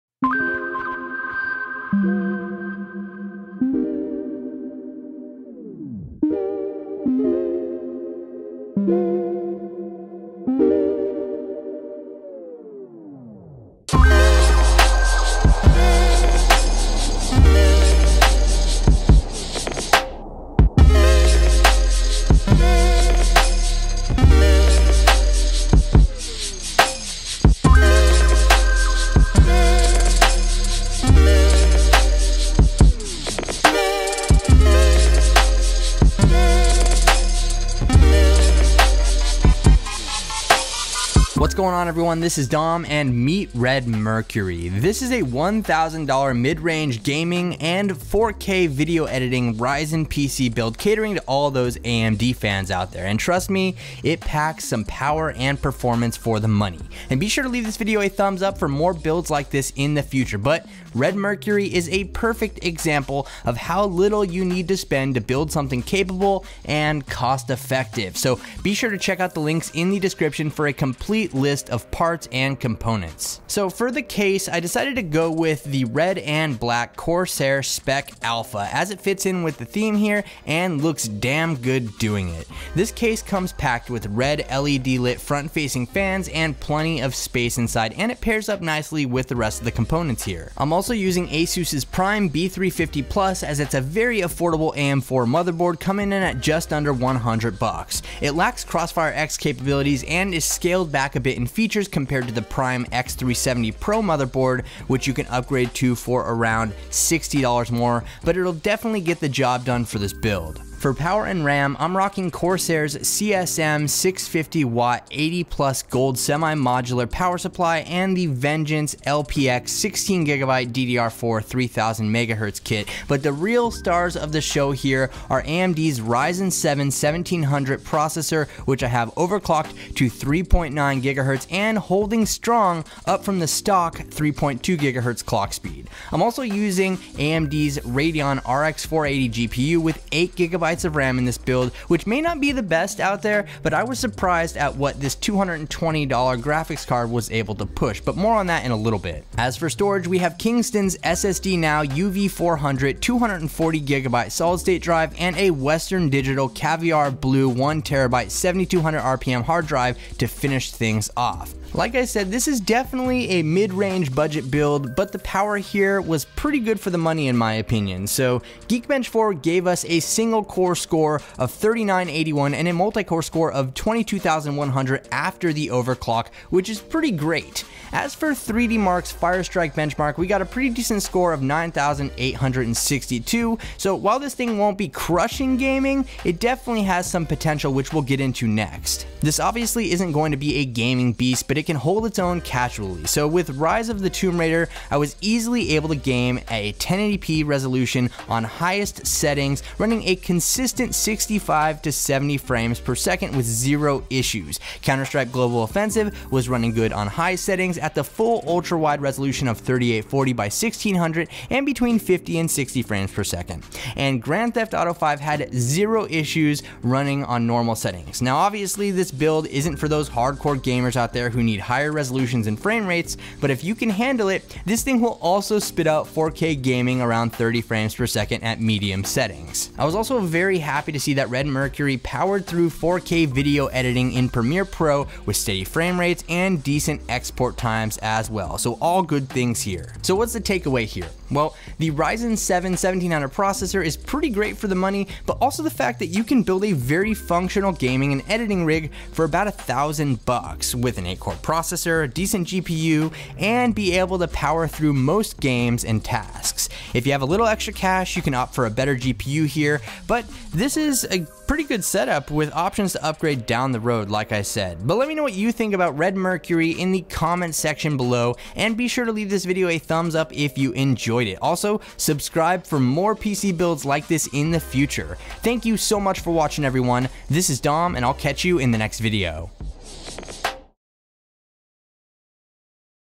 Build The Best $1000 Ryzen Gaming PC! (April 2017)
AMD Ryzen 7 (1700) Gaming PC Build for $ 1000 - April 2017 - This AMD Ryzen 7 (1700) PC build is great for gaming and 4K video editing. This red and black themed Ryzen build is budget at just $1,000. This features the RX480 and Ryzen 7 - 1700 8-core processor!

▶FULL PART LIST -